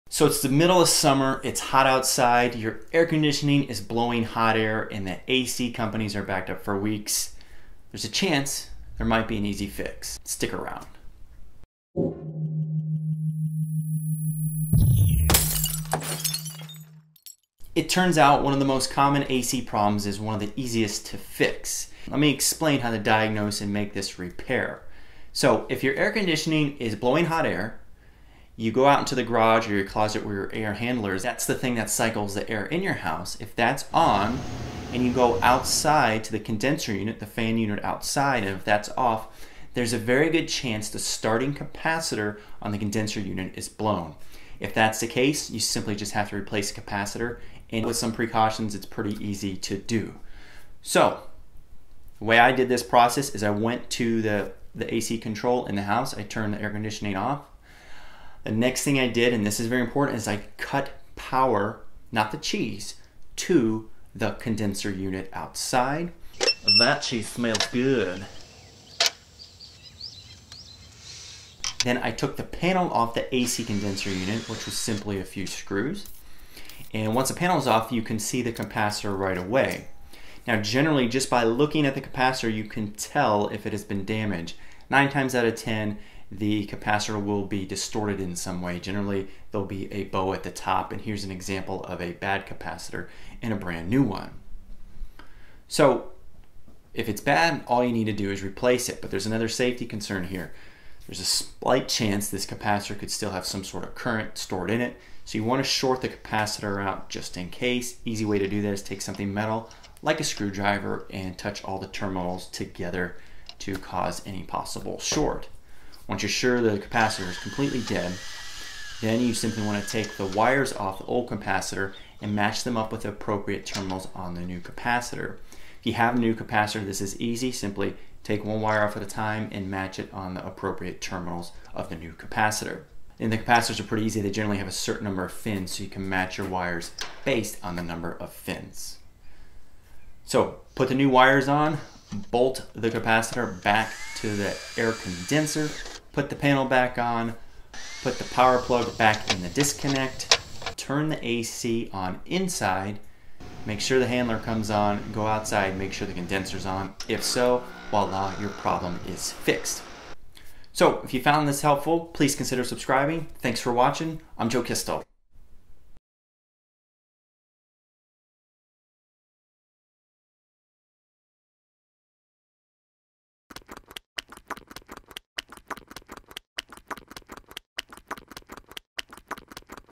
How To Replace Air Conditioner Condenser Capacitor
A quick and easy way to repair an air condition that is not blowing cold air.    Is your air condition on but blowing hot air? Is the outside fan unit off while the inside blower unit is on?  If so it is a common AC problem that simply require the change of one part that is very easy to do if you have the part.

Replacement Capacitor
(worked for the Trane XR14 Condenser Unit)

Nut Driver Set
(to remove condenser panel)

This video shows how to diagnose the common problem of a faulty starting capacitor on an air condenser unit (the outside fan box) and how to replace it.  The system in the video is a Trane XR14 unit but the procedure is likely similar to other brands of air conditioning systems. It is an easy task to replace a bad capacitor in an air condition unit with the proper precautions.  It is viral electricity is shut off to the condenser unit and the capacitor shorted prior to handling the starting capacitor.